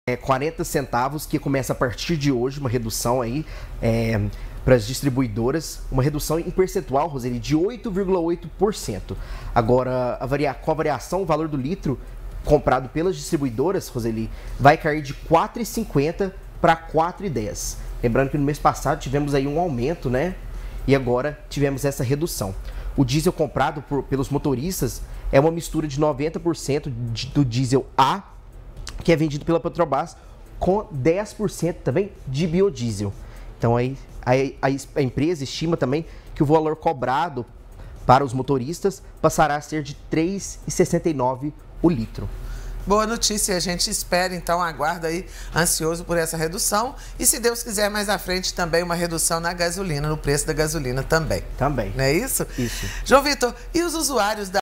PETROBRAS ANUNCIA REDUÇÃO DE 40 CENTAVOS NO LITRO DO DIESEL
| BRASIL |

A PETROBRAS ANUNCIOU A REDUÇÃO DE 40 CENTAVOS NO LITRO DO DIESEL PARA AS DISTRIBUIDORAS.